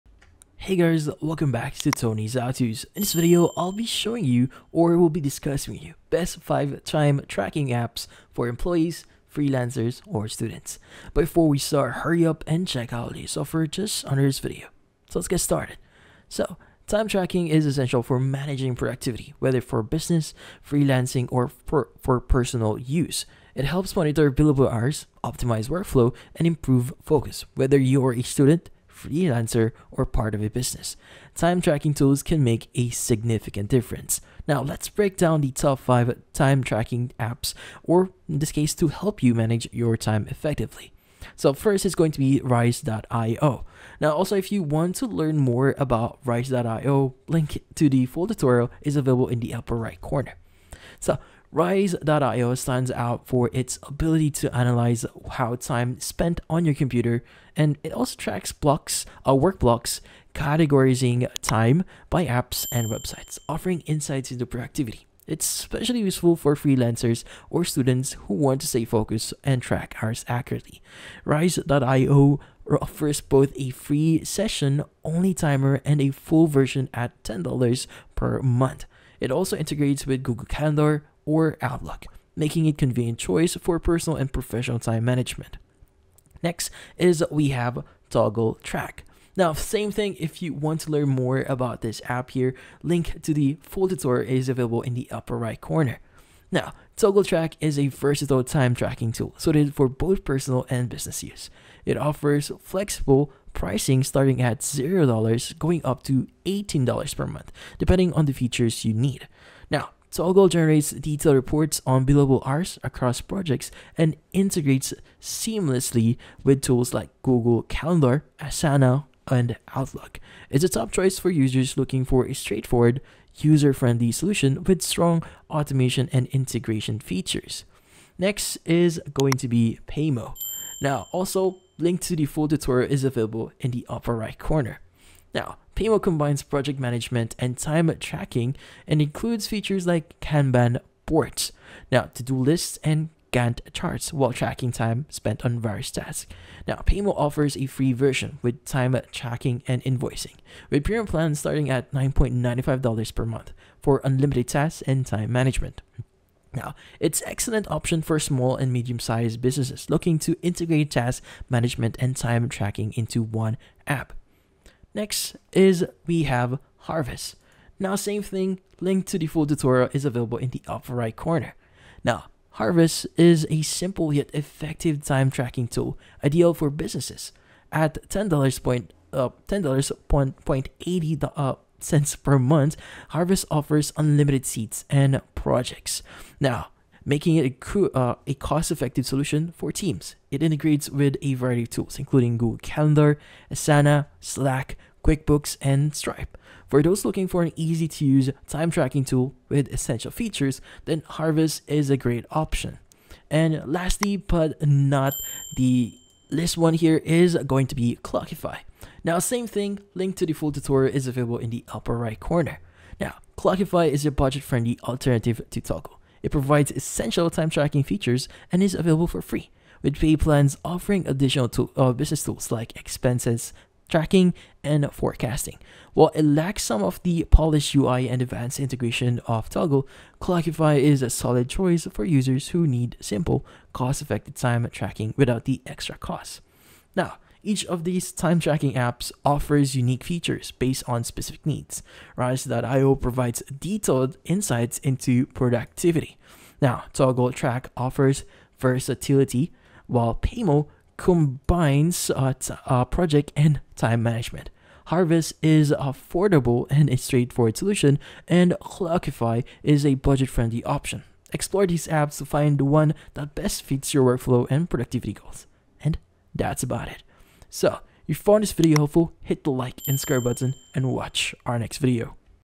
Best 5 Time Tracker Apps (Employees, Freelancers, Students, 2024)
In this video, I show you Best 5 Time Tracker Apps (Employees, Freelancers, Students) in 2024. The quick and easy tutorial how to manage your time better with Toggl Track, Raize Ai, Clockify, Harvest, paymo. Do you want to know step by step best time tracker app for students in 2024? Yes? Great!